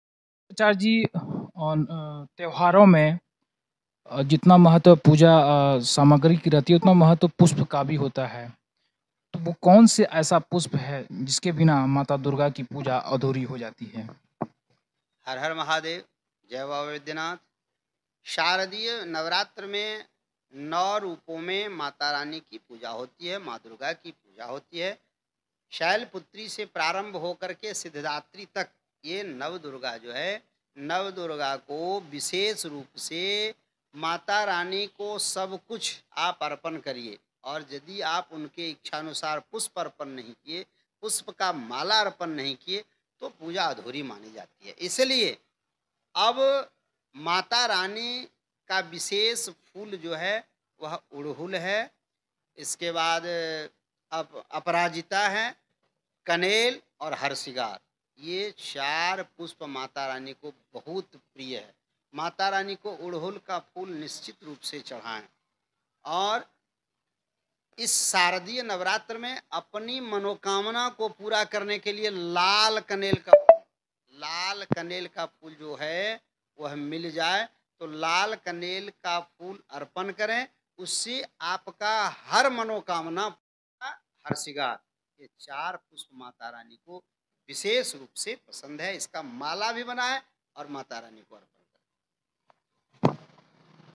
Shardiya Navratri 2024: देवी पूजन में कौन से फूल हैं जरूरी? ज्योतिषाचार्य से जानें
This year, Shardiya Navratri has started from 3rd October and Navratri is going to end with Vijayadashami on 12th October. During Navratri, Navdurga is worshiped with rituals. By worshiping Navdurga one attains success, strength and intelligence.
इस साल शारदिया नवरात्रि की शुरुआत 3 अक्टूबर से हो चुकी है और नवरात्रि का समापन 12 अक्टूबर को विजयदशमी के साथ ही होने वाला है. नवरात्र में नवदुर्गा की पूजा आराधना विधि विधान के साथ की जाती है. नवदुर्गा की पूजा आराधना करने से सिद्धि,बल, बुद्धि की प्राप्ति होती है.